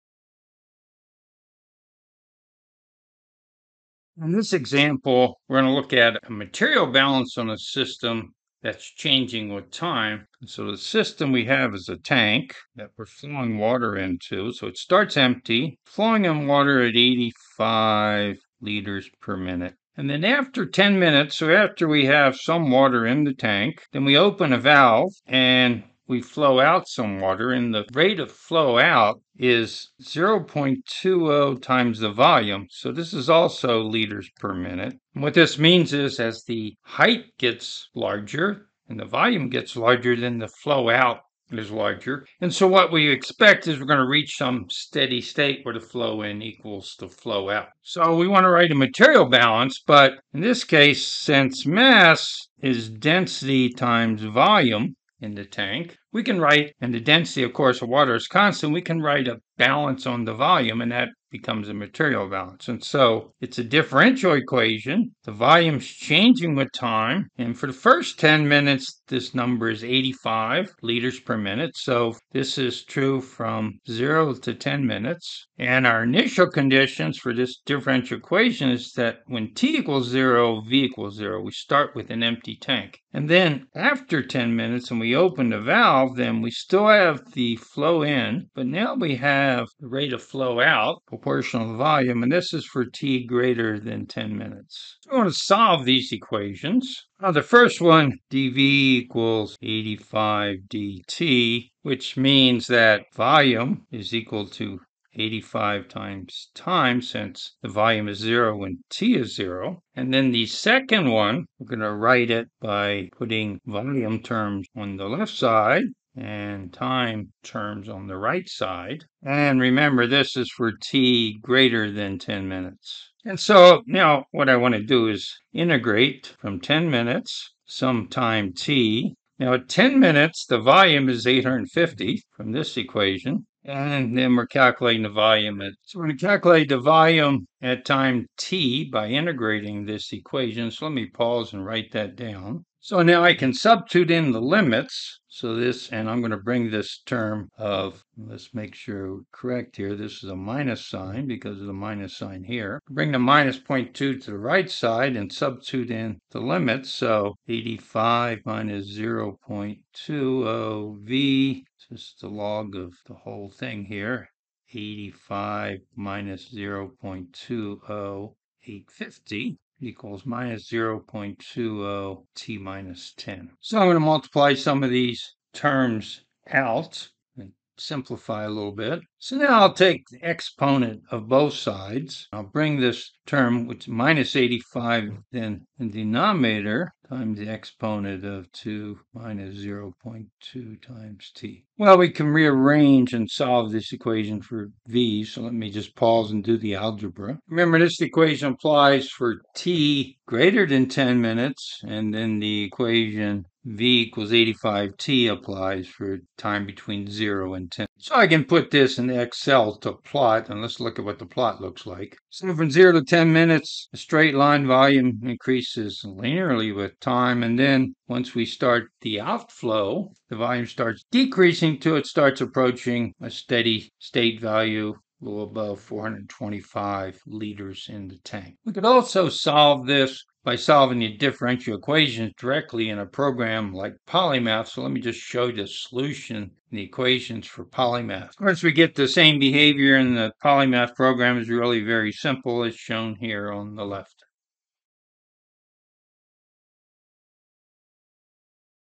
Transient Material Balance Example
Organized by textbook Material balance for water flowing into and out of a tank. The ordinary differential equations are solved analytically and numerically with Polymath. Made by faculty at the University of Colorado Boulder, Department of Chemical & Biological Engineering.